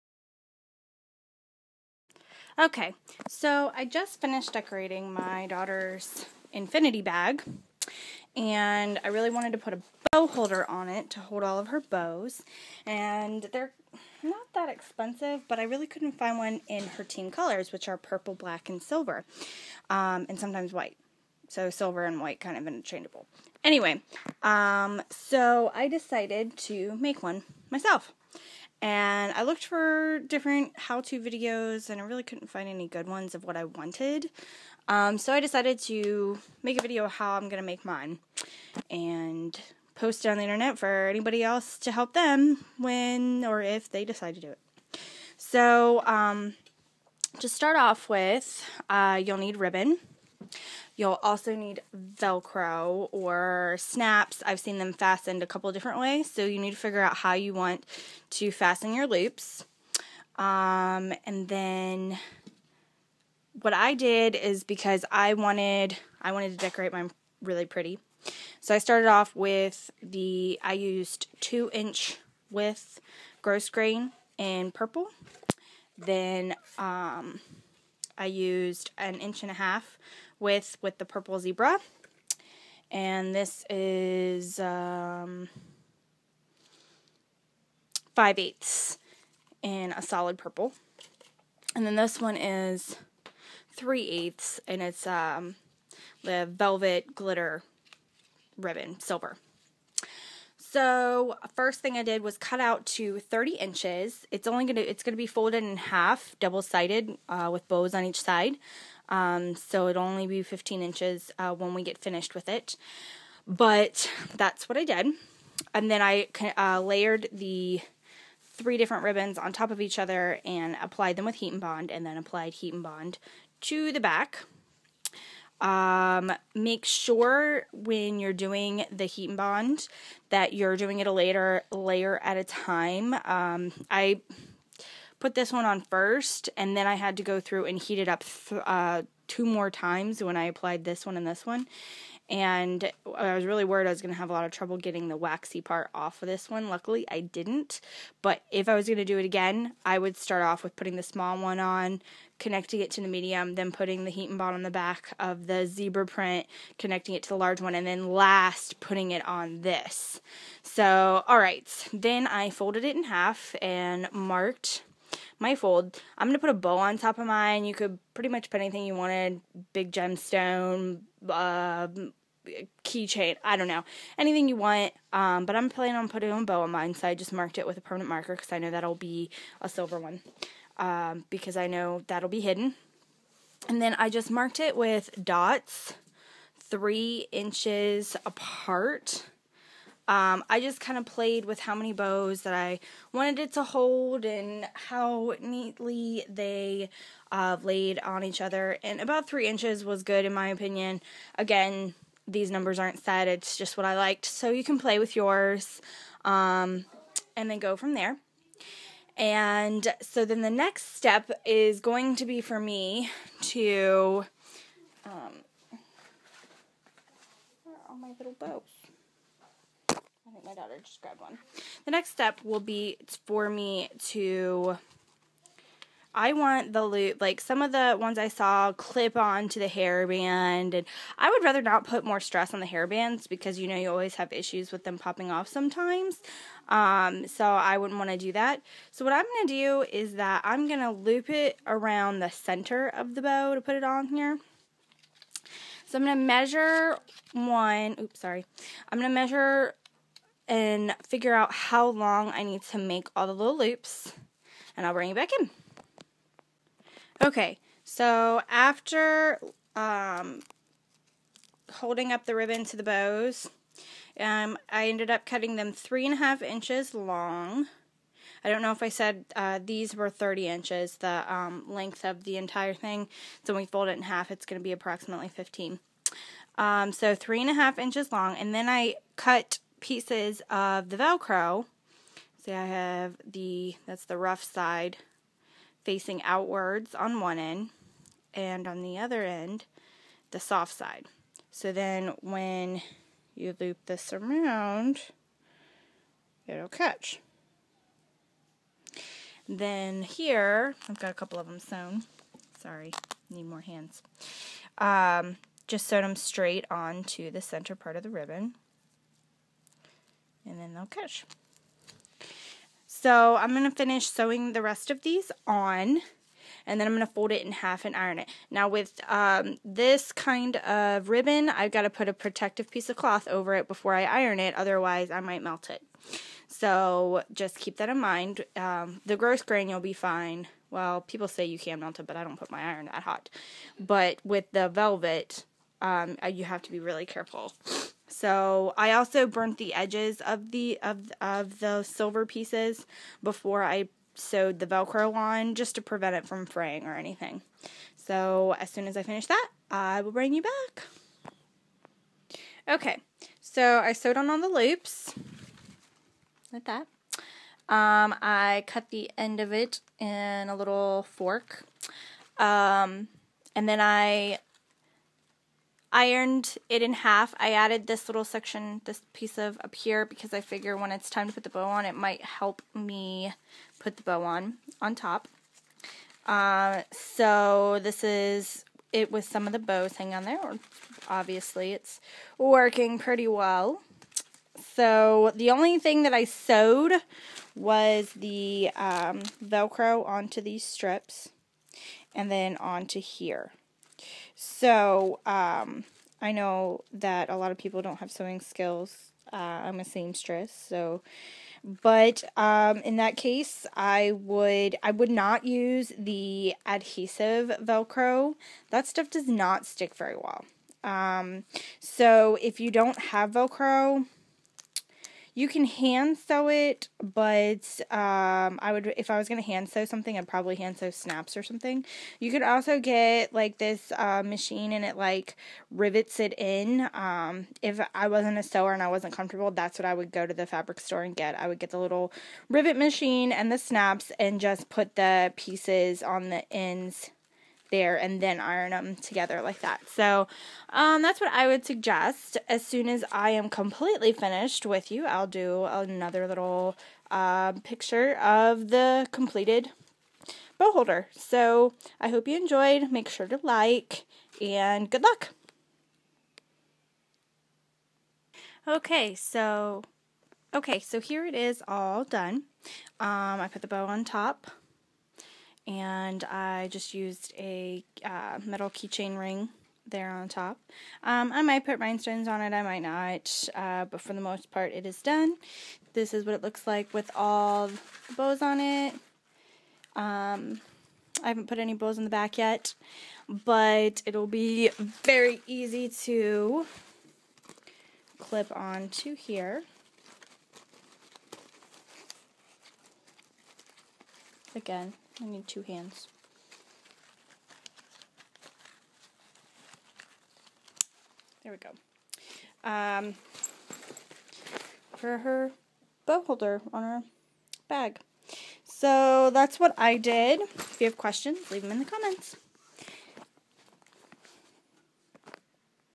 DIY Cheer Bow holder Backpack Strip
Made my daughter a backpack strip Cheer bow holder. Thought I would make a video of how I did it :)